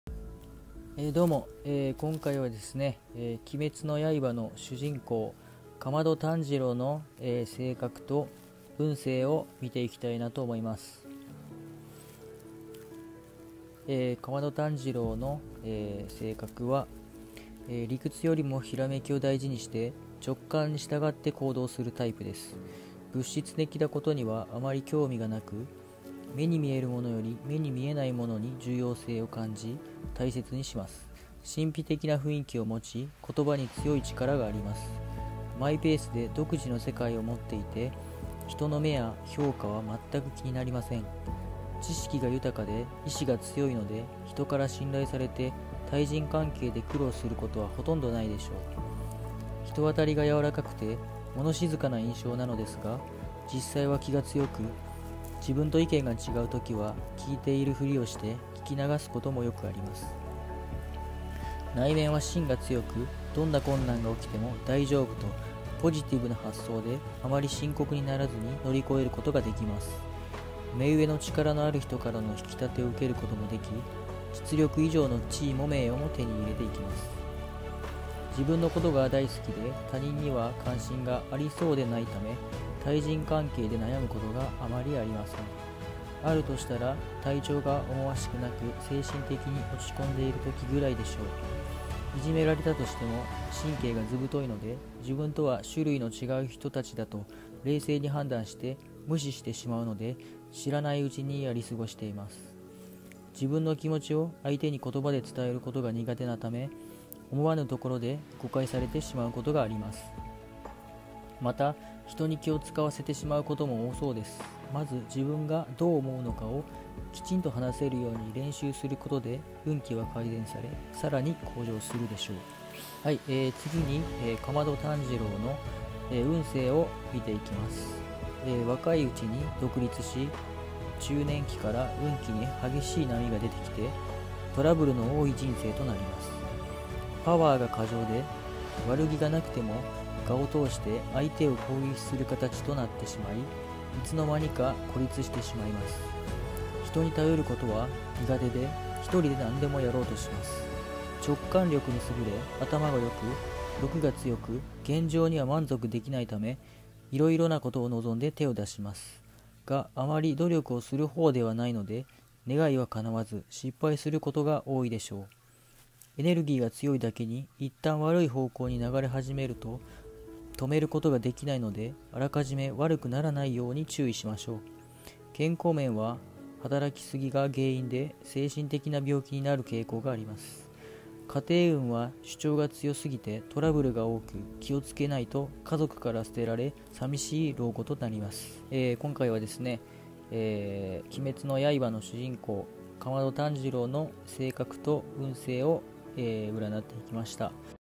【鬼滅の刃】竈門炭治郎の性格と運勢を占ってみた
今回は鬼滅の刃の竈門炭治郎の性格と運勢を占ってみました。